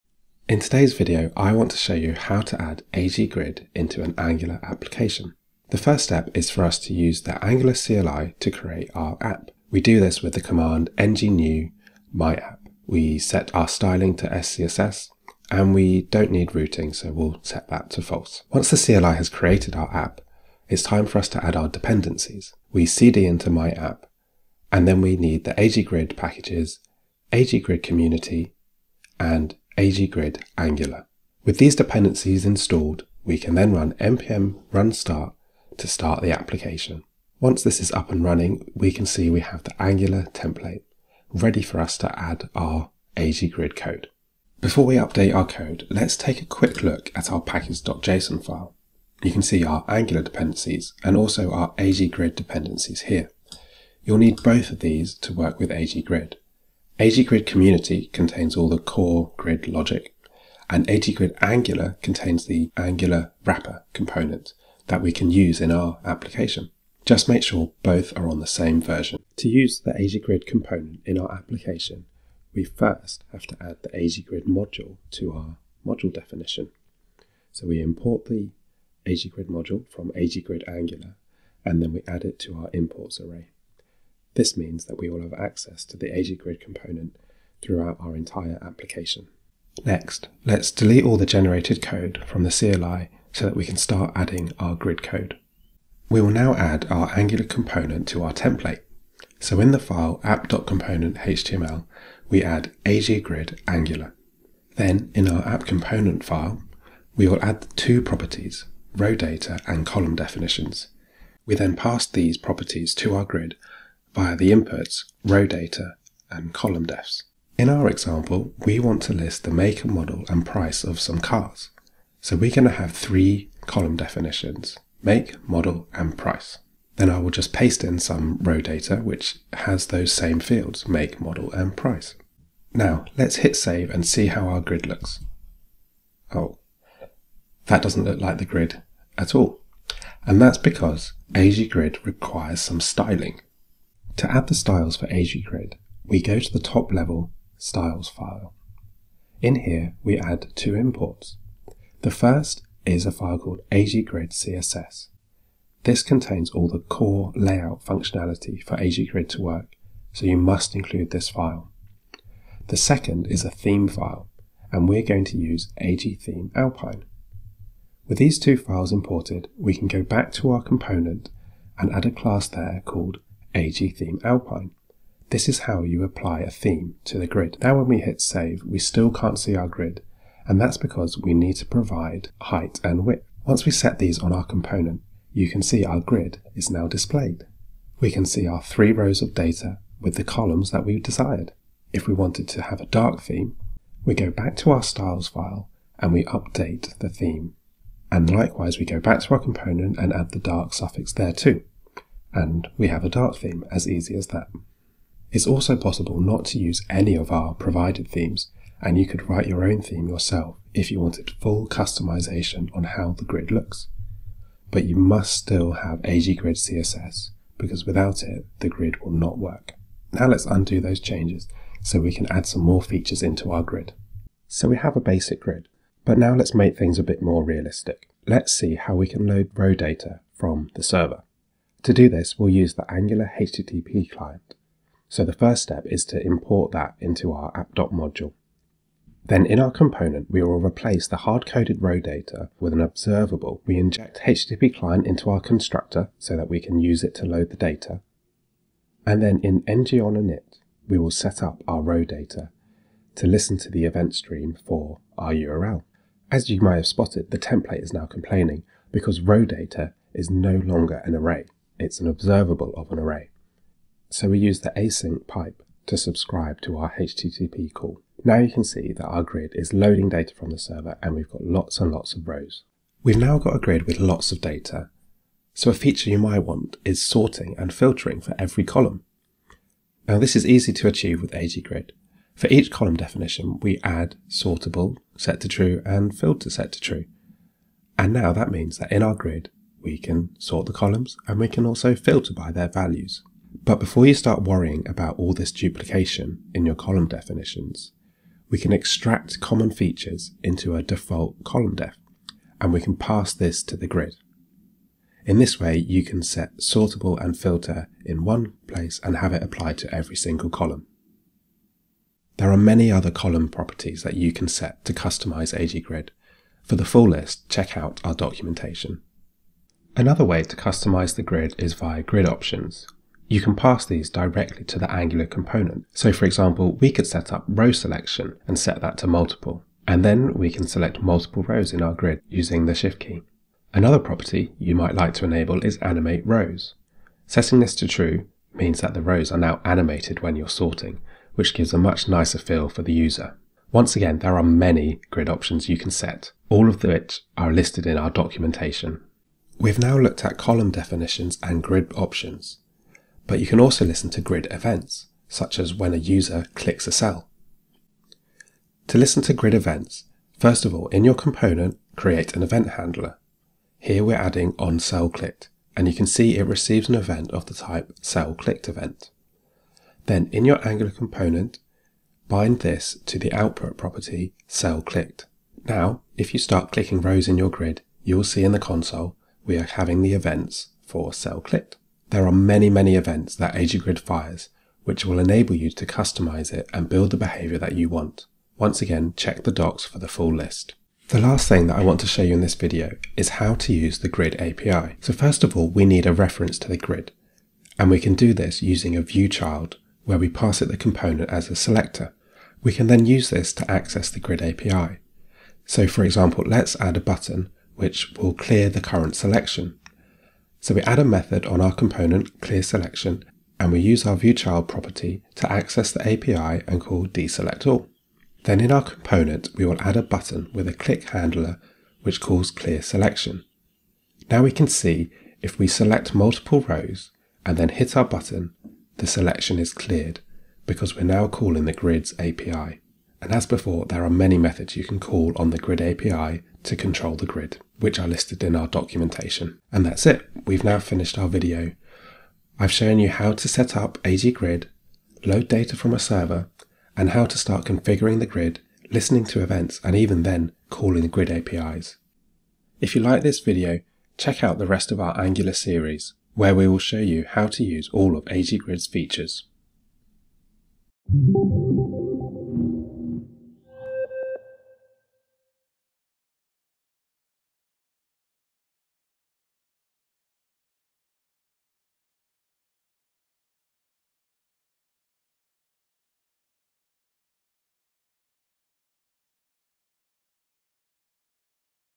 Getting Started With Angular and AG Grid
In this video Stephen Cooper provides a QuickStart Guide to getting started with Angular and AG Grid where you will learn how to render hard coded data, data from a server, using column and grid properties and custom code to handle events.

00:00 Create Angular App
01:13 Using AG Grid
03:45 Loading Data via HTTP
04:40 Sorting and Filtering
05:20 Grid Options
05:50 Grid Events
06:30 Grid API
07:20 Summary

AG Grid works with React, Angular, Vue, and JavaScript. Available as a free community edition and commercially as an Enterprise product (full support and more features).